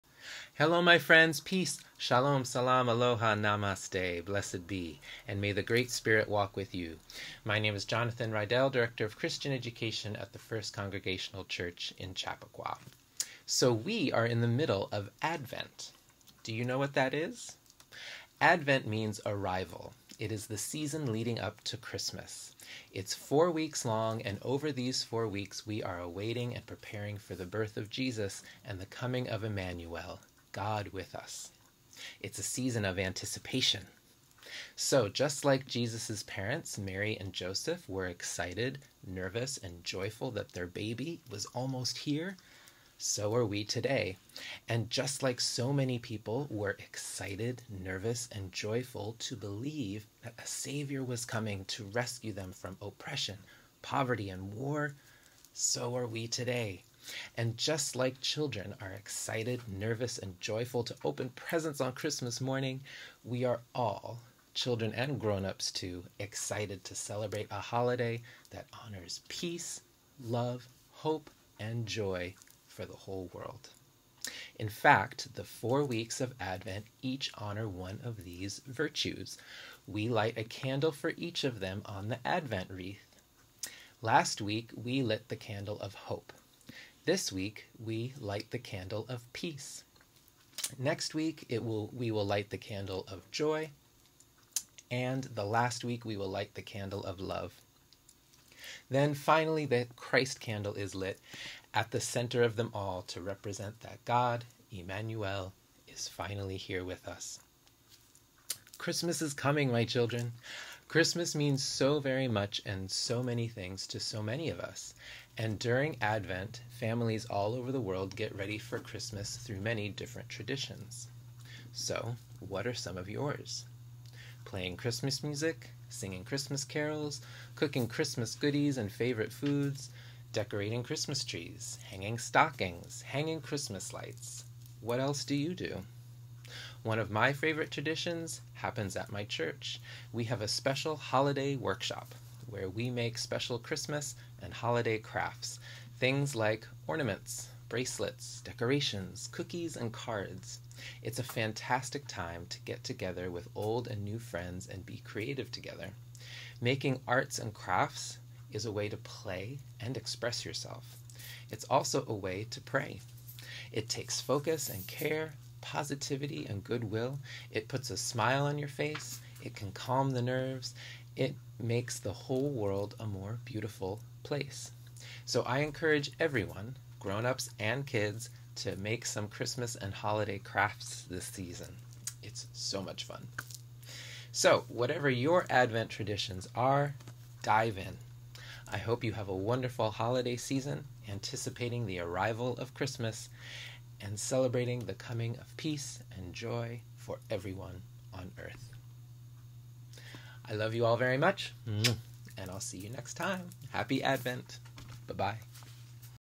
Youth Message: Advent - Christmas is Coming! 12.10.2023
It’s Advent!  The season leading up to Christmas! It’s a season of anticipation! Over these four weeks are awaiting and preparing for the big day! But what does it mean to anticipate something?  What is it about the story of Christmas that has us waiting and preparing?  What does it mean to live in a season of Love? Let’s talk more about this!